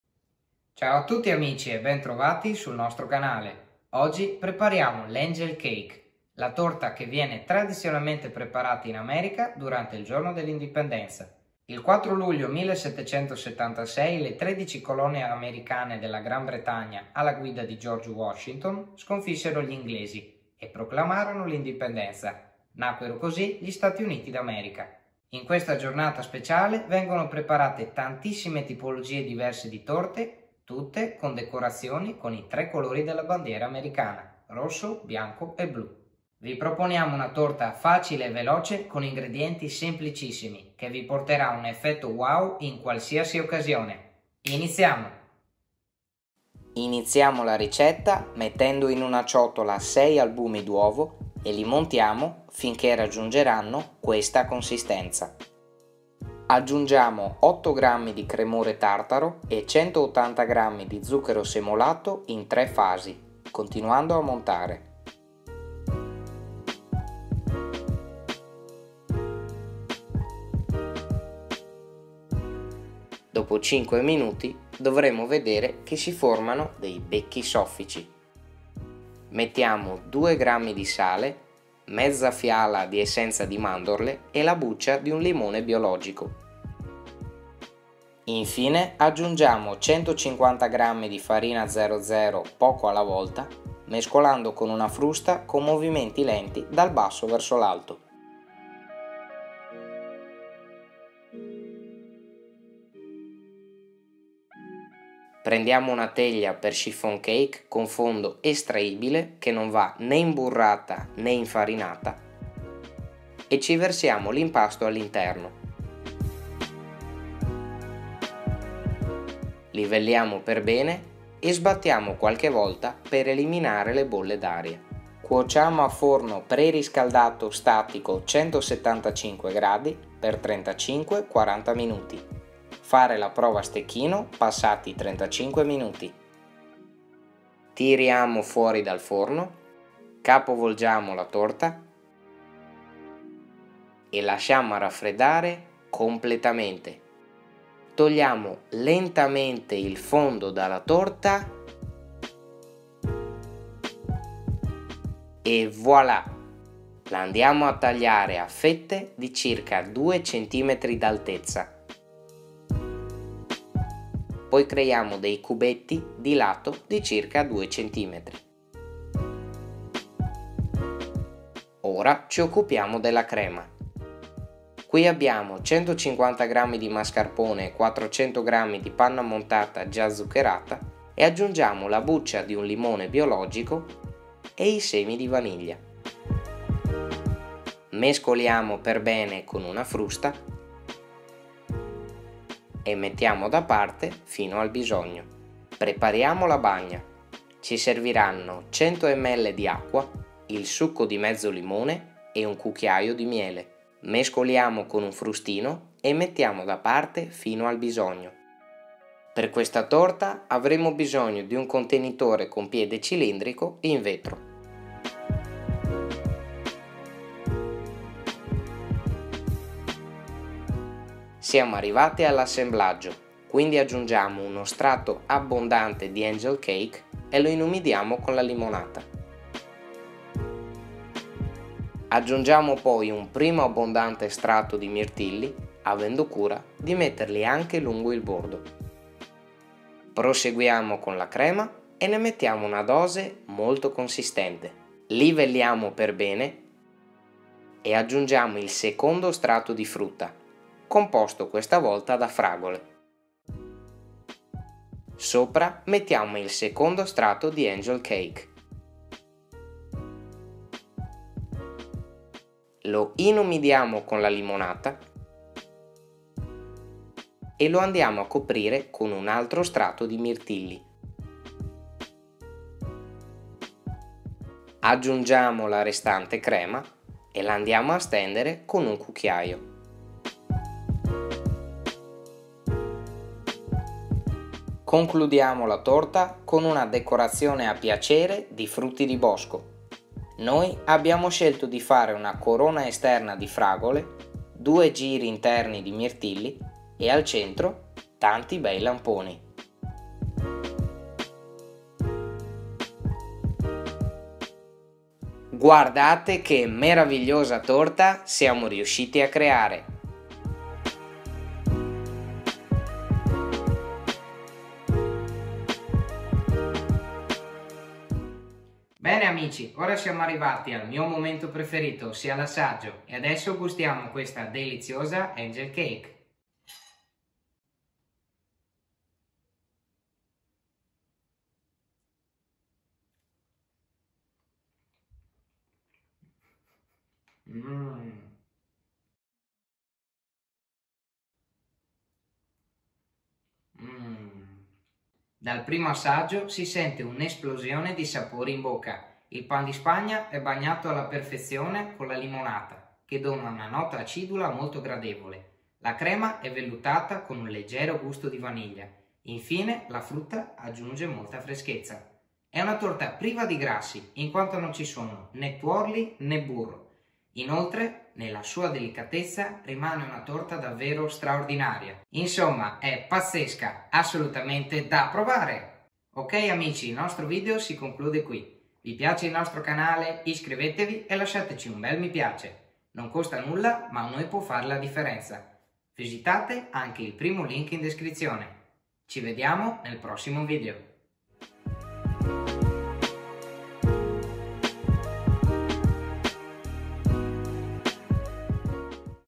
ANGEL CAKE facile e veloce
Per una teglia di 20 cm di diametro e 10 cm d'altezza:
0:46 ANGEL CAKE (trifle cake)
6 albumi
180 g di zucchero semolato
8 g di cremore tartaro
2 g di sale
150 g di farina "00"
la buccia di mezzo limone
mezza fiala di essenza di mandorle
Cottura : forno statico preriscaldato, 175°C per 35-40 minuti. Fare la prova stecchino passato i 35 minuti.

2:55 CREMA
150 g di mascarpone
400 g di panna montata già zuccherata
la scorza di un limone biologico
i semi di una bacca di vaniglia Bourbon